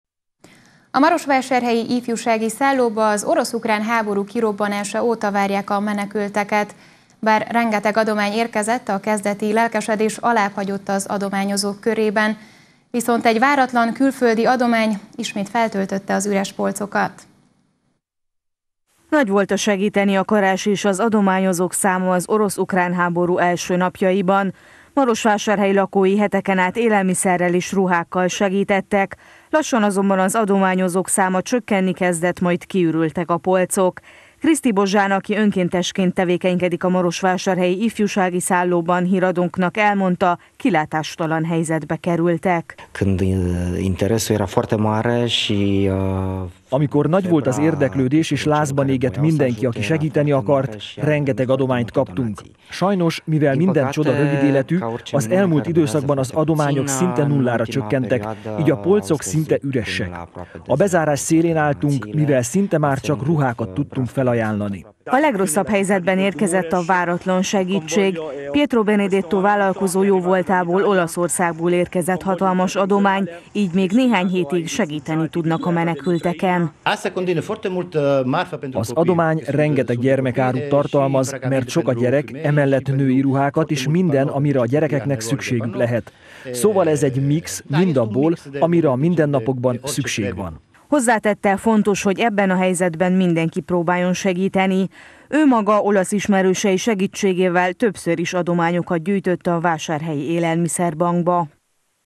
Váratlan segítség érkezett – Erdélyi Magyar Televízió
A marosvásárhelyi Ifjúsági Szállóba az orosz-ukrán háború kirobbanása óta várják a menekülteket. Bár rengeteg adomány érkezett hetekkel ezelőtt, a kezdeti lelkesedés alábbhagyott az adományozók körében, így egy váratlan külföldi adomány töltötte fel ismét az igencsak üres polcokat.